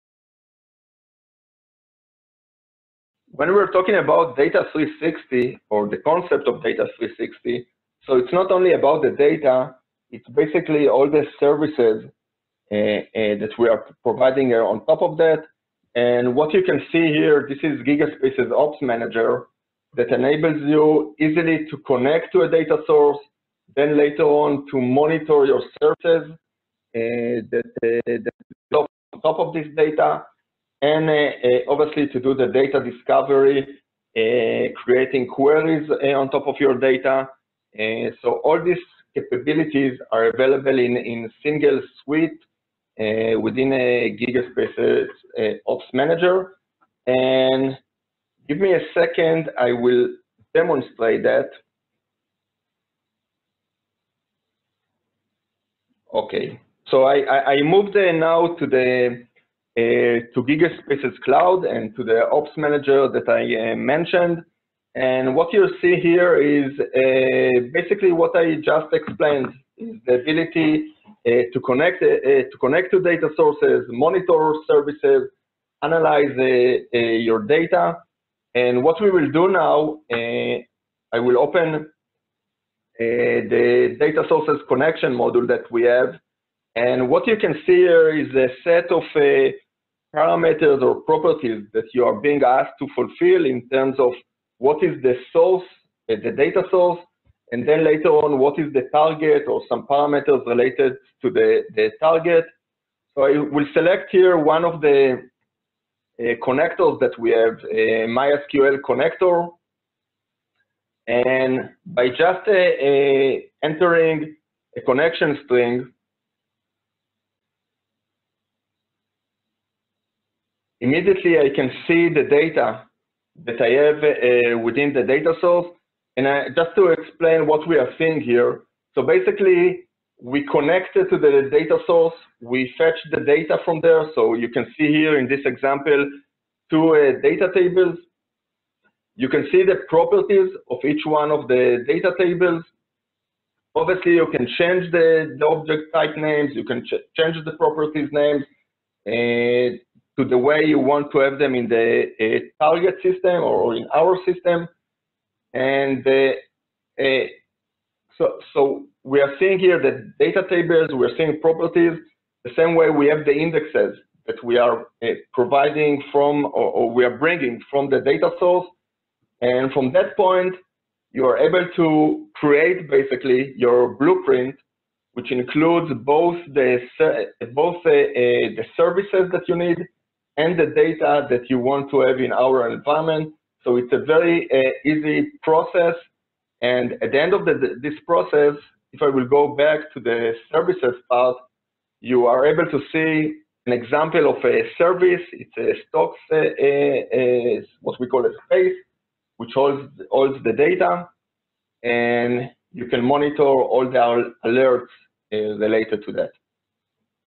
GigaSpaces Ops Manager & Data Source Connectivity Demo
GigaSpaces InsightEdge Platform is now available as a service on the Google Cloud Platform. GigaSpaces Cloud enables users to easily access InsightEdge Platform by creating an account on-the-fly, in a managed cloud environment. It also allows users to leverage business logic, hot and warm and cold tiering, data lake indexing and BI acceleration.

In this tutorial, we will show you how to capture data from any source for a real-time 360 view with GigaSpaces Cloud, and instantly leverage the benefits of the GigaSpaces analytics platform — fast performance, scalability, and low-latency for analytics and machine learning model execution:

* One-click deployment on any cloud
* Zero management overhead
* Fast analytics, BI and visualization
* 360 real-time view of all data, across any data source